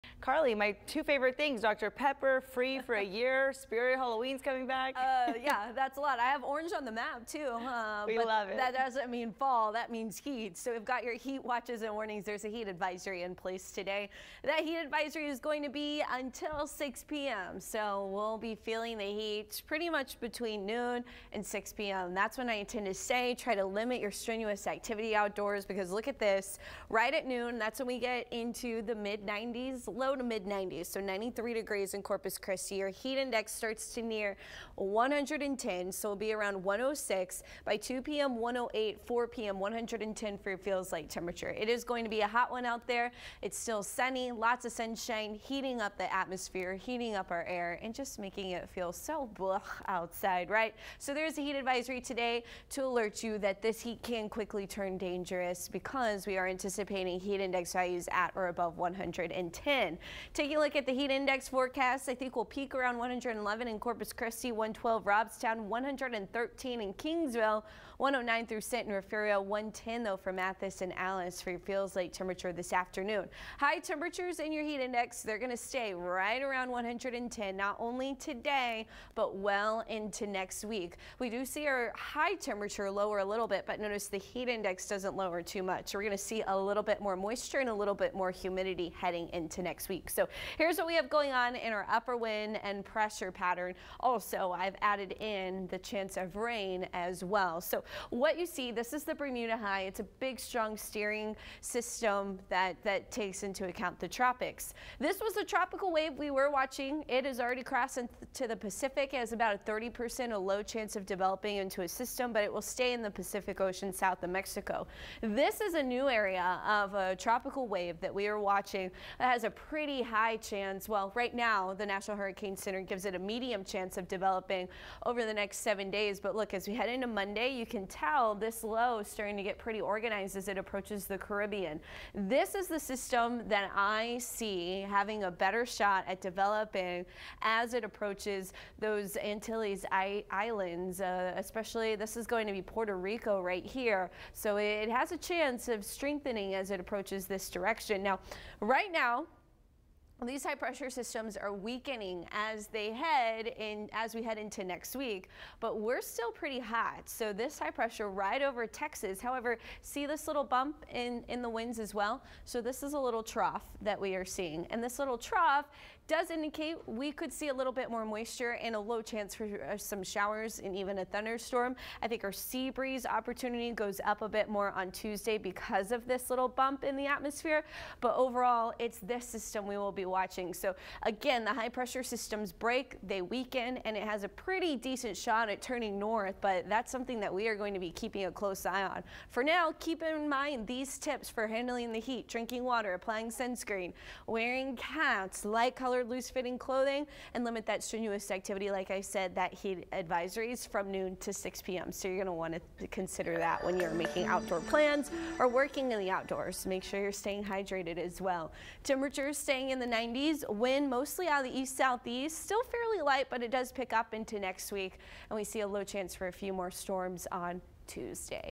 High pressure systems weakening in the tropics
We are keeping an eye on one high pressure system that has a decent shot of turning north.